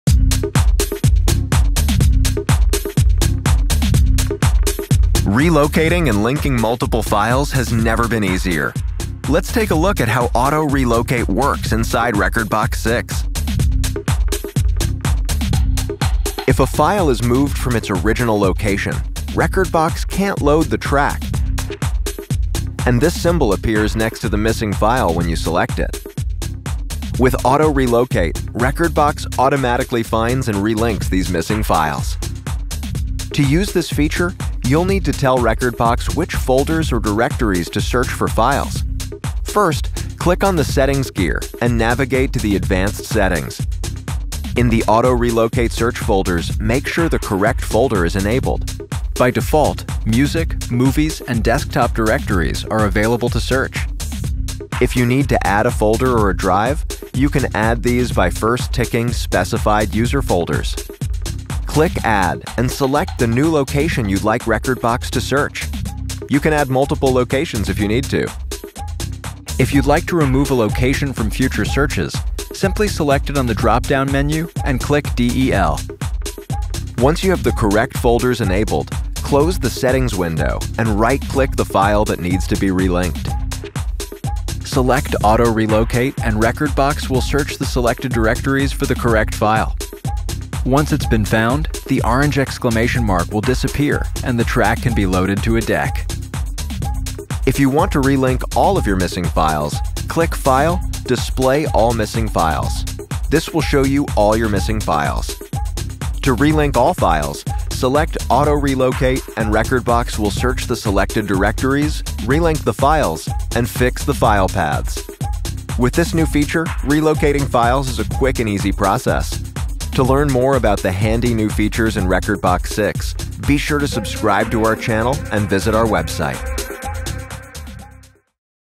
How to use Auto Relocate | Tutorials - rekordbox ver. 6.0 (and after)
📍 Discover the NEW rekordbox ver.

Never “lose” a track again thanks to the Auto Relocate feature in rekordbox.

If the track you want to play next is missing because you moved it, rekordbox will search all locations within a folder chosen by you (e.g., your Pioneer DJ folder) until it finds the track, corrects the path, and plays it from the new location.

# CLOUD-CONNECTED